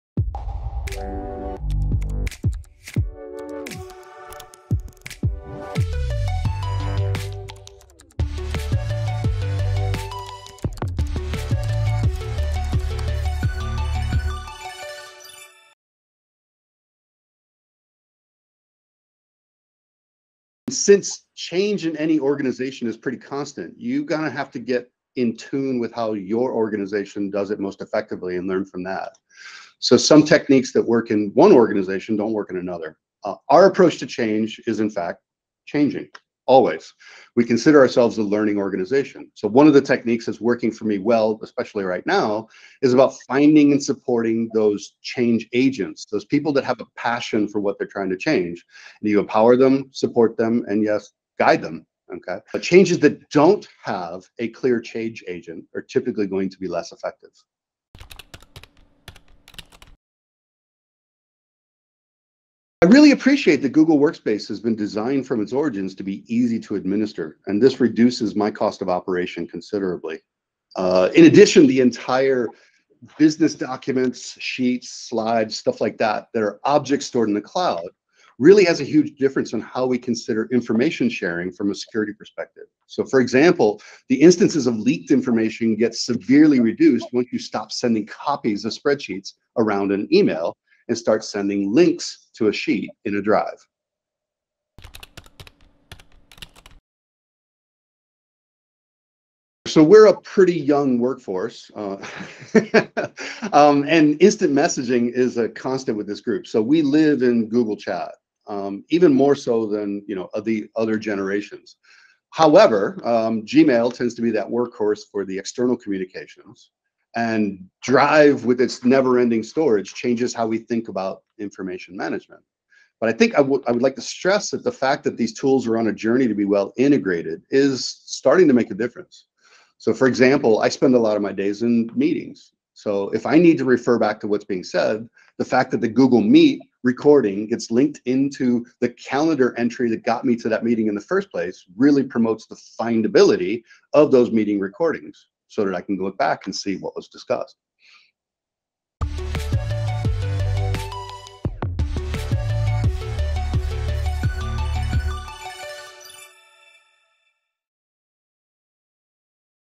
Google Workspace: Champions of Change: aCommerce
Google Workspace APAC’s Champion of Change, Mark Rachelski, Group CTO shares how Google Workspace enables easy admin, reduces cost, and improves collaboration and communication for aCommerce.